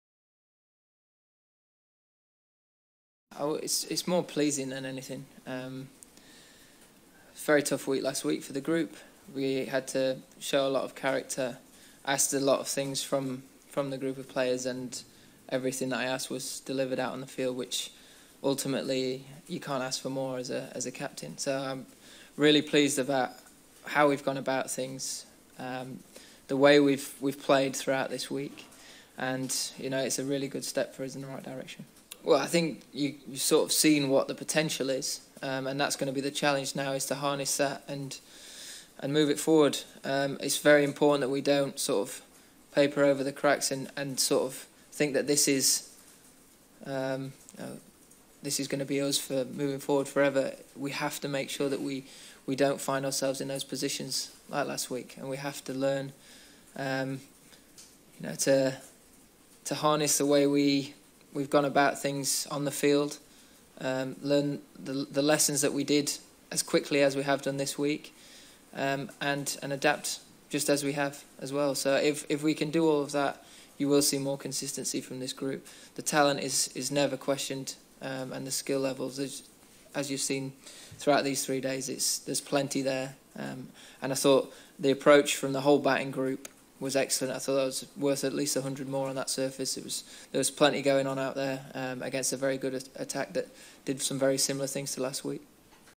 Everything I asked was delivered on the field - Root
Joe Root challenges his England side to maintain the level of performance from their innings victory over Pakistan at Headingley